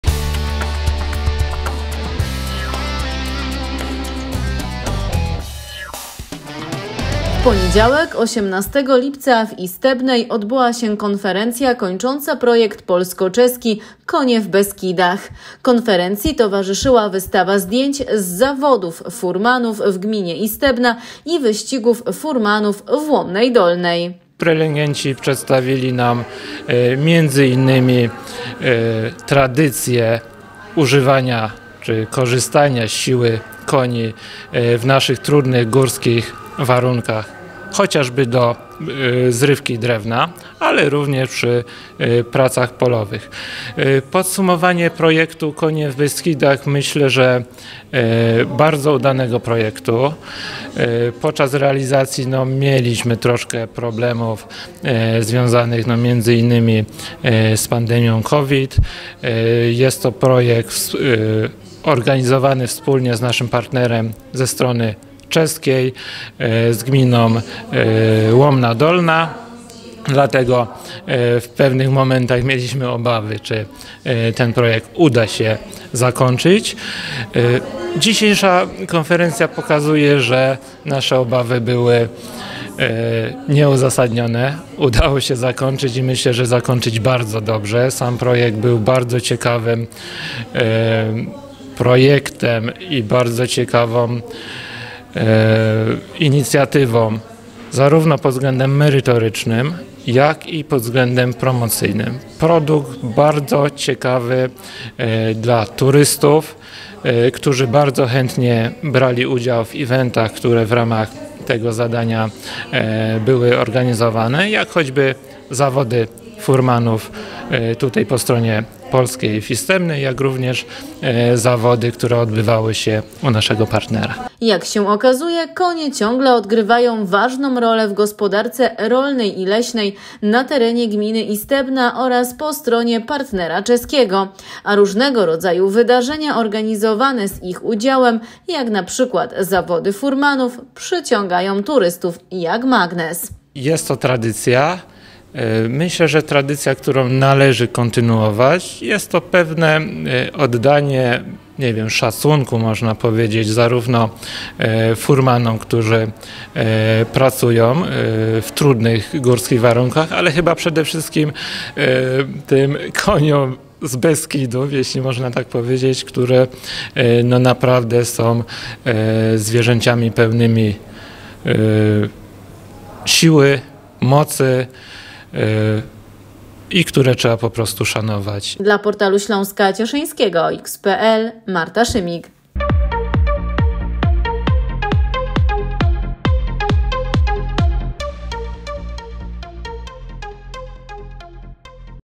Konie w Beskidach
18 lipca w Istebnej odbyła się konferencja kończąca projekt polsko-czeski pn. "Konie w Beskidach/Koně v Beskydech"  wraz z wystawą zdjęć z zawodów furmanów w Gminie Istebna i wyścigów furmanów w Łomnej Dolnej. Konferencja miała na celu podsumowanie projektu, ocenę działań oraz wyciągnięcie wniosków na przyszłość.